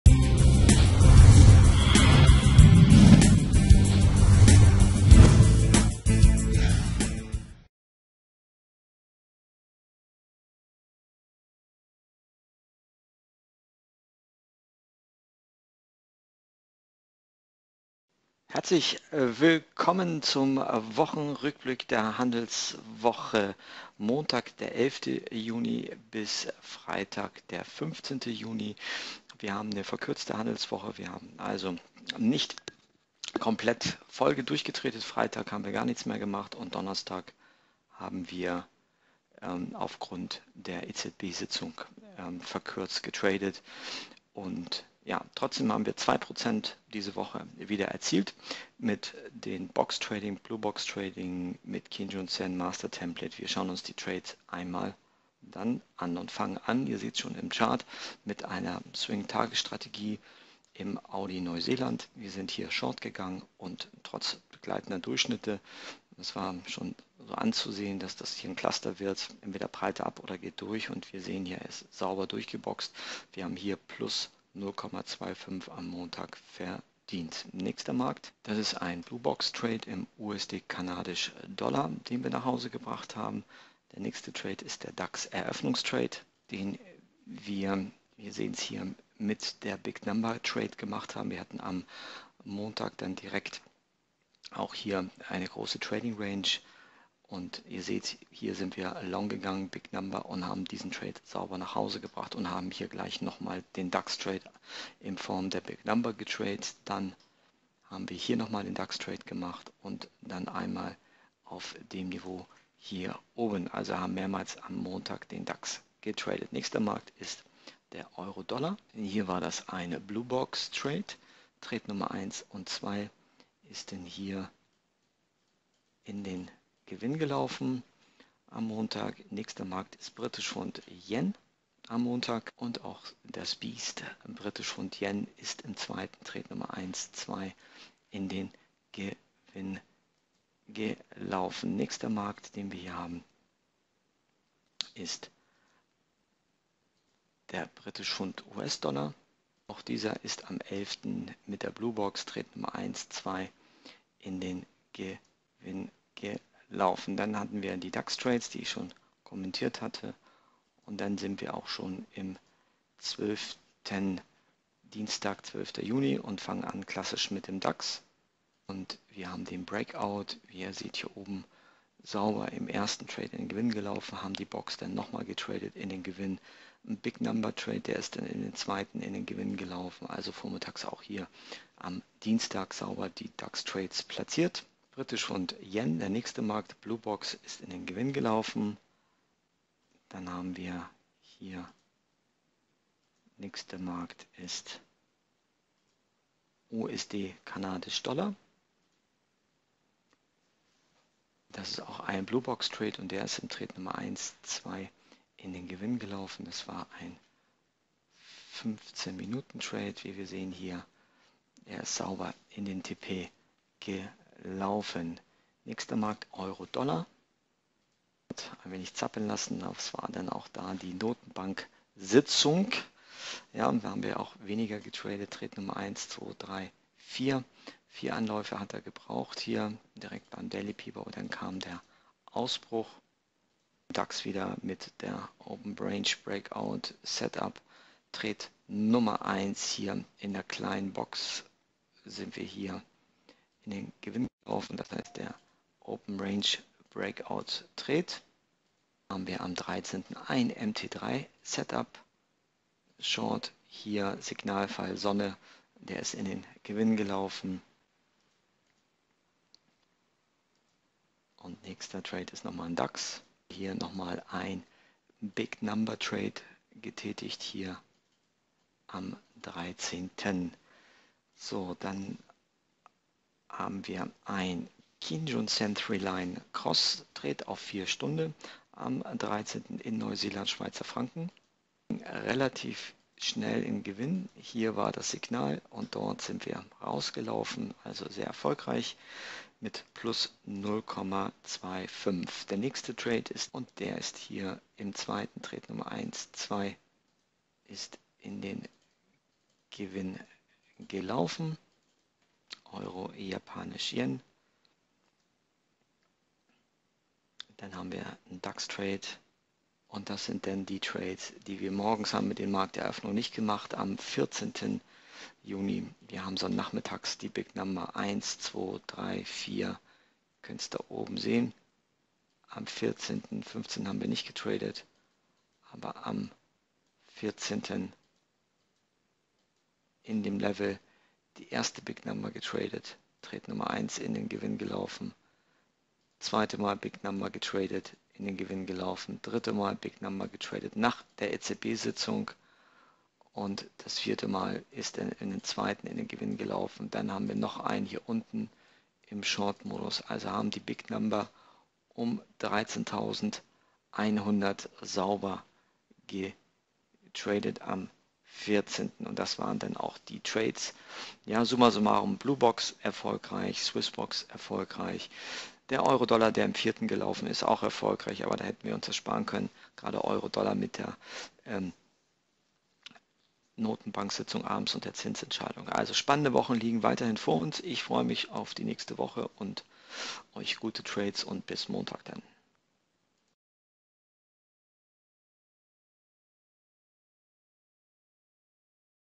Wochenrückblick der SwissBox Trades vom 11.-15.06.2018
Carlos Martins zeigt im Wochenrückblick die SwissBox Trades (Open Range Breakout) vom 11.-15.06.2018.